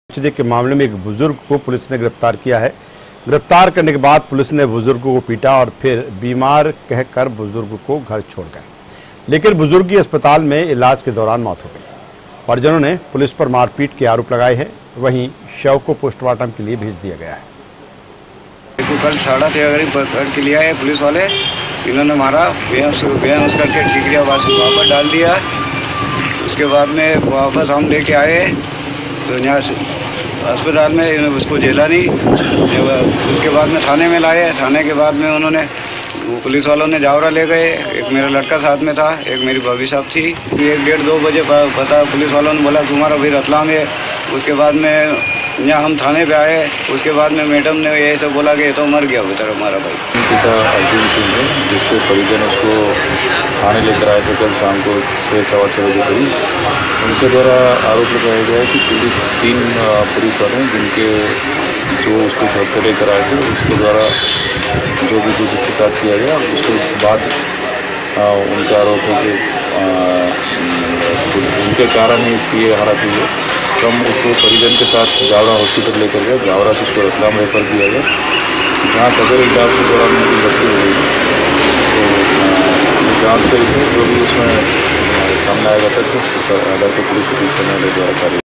बुजुर्ग को पुलिस ने पीटा Ratlam | Madhya Pradesh
रतलाम जिले के ठिकरिया गांव में कच्ची शराब बेचने के मामले में एक बुर्जुग को पुलिस ने गिरफ्तार किया...गिरफ्तार करने के बाद पुलिस ने बुजुर्ग को पीटा...और फिर बीमार कहकर बुजुर्ग को घर छोड़ गए. लेकिन बुजुर्ग की अस्पताल में इलाज के दौरान मौत हो गई...परिजनों ने पुलिस पर मारपीट के आरोप लगाए हैं...वहीं शव को पोस्टमार्टम के लिए भेज दिया है...